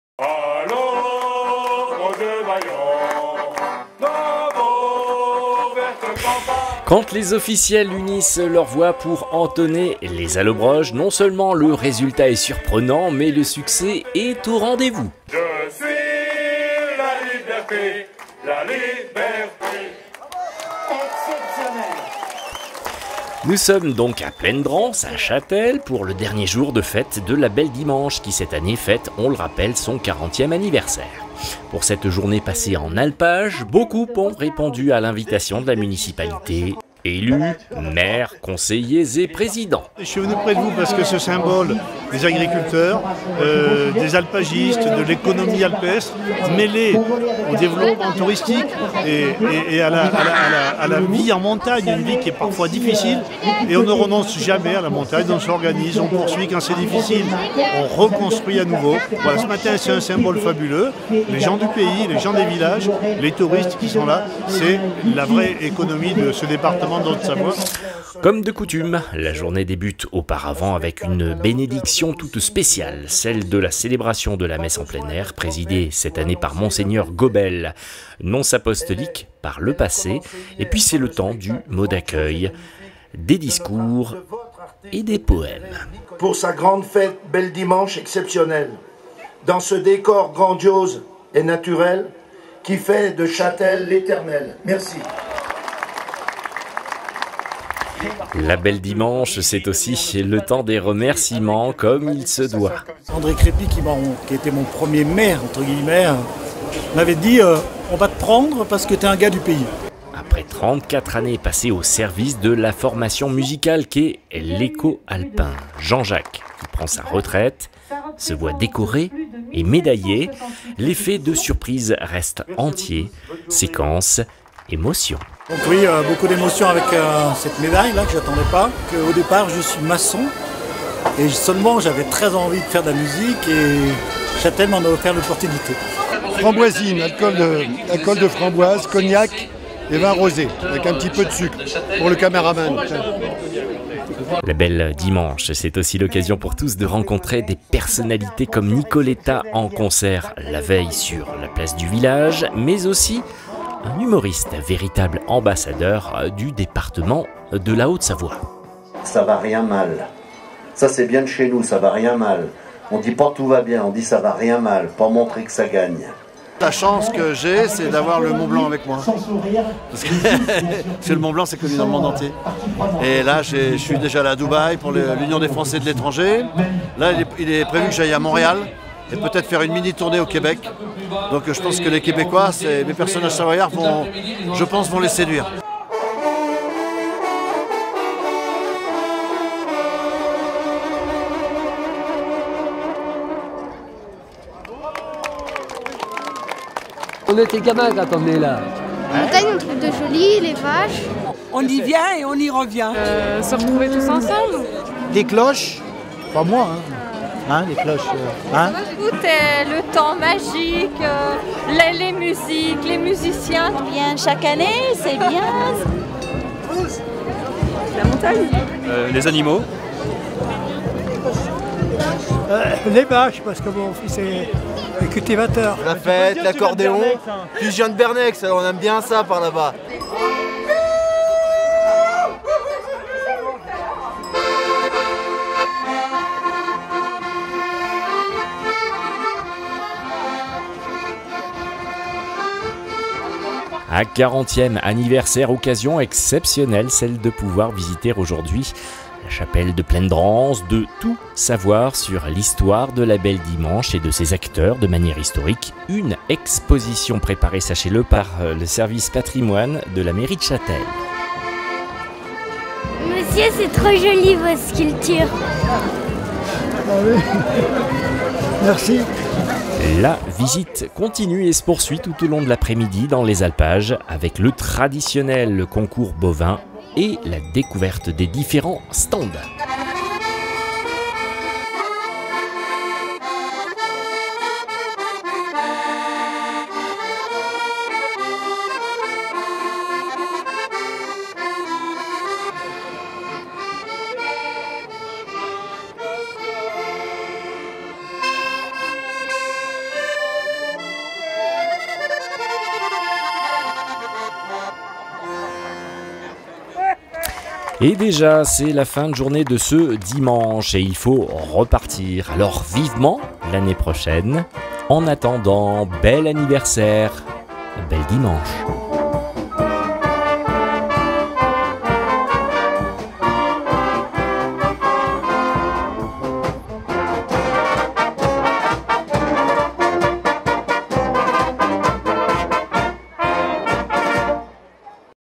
La Belle Dimanche : les Alpages...
A J+3 après événement, suite et fin de notre zoom sur la Belle Dimanche et son 40° anniversaire. Au programme de la journée passée à Plaine Dranse : stands, agriculteurs, concours, musique et festivités en tous genres...